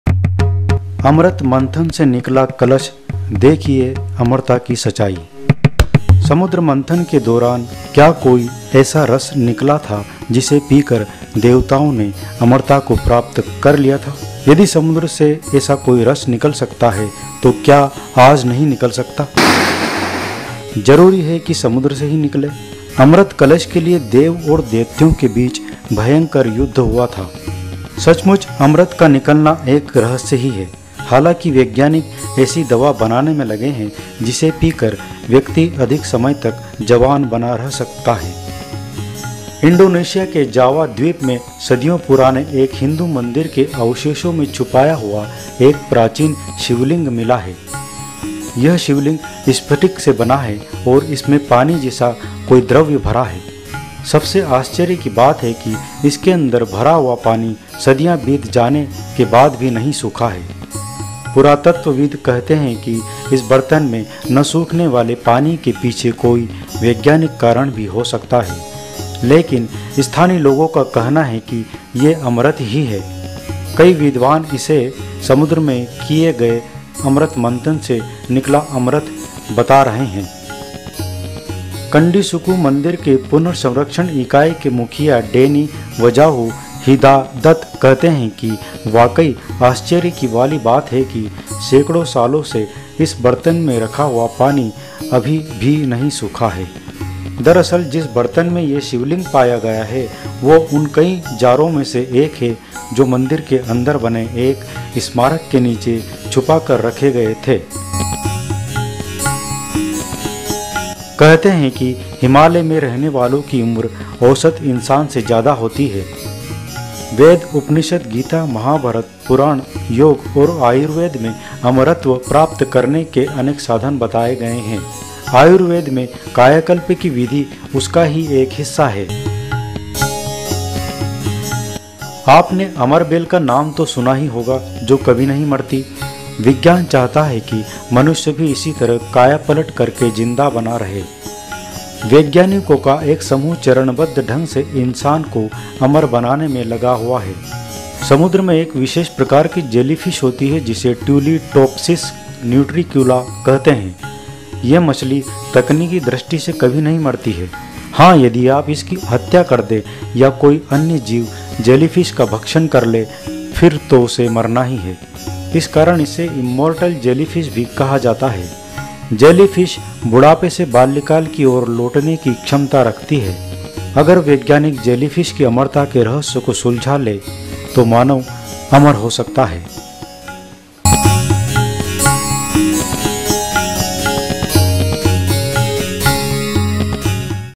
अमृत मंथन से निकला 'कलश' देखिए अमरता की सचाई...
समुद्र मंथन के दौरान क्या कोई ऐसा रस निकला था जिसे पीकर देवताओं ने अमरता को प्राप्त कर लिया था? यदि समुद्र से ऐसा कोई रस निकल सकता है तो क्या आज नहीं निकल सकता? जरूरी है कि समुद्र से ही निकले? अमृत कलश के लिए देव और दैत्यों के बीच भयंकर युद्ध हुआ था। सचमुच अमृत का निकलना एक रहस्य ही है। हालांकि वैज्ञानिक ऐसी दवा बनाने में लगे हैं जिसे पीकर व्यक्ति अधिक समय तक जवान बना रह सके।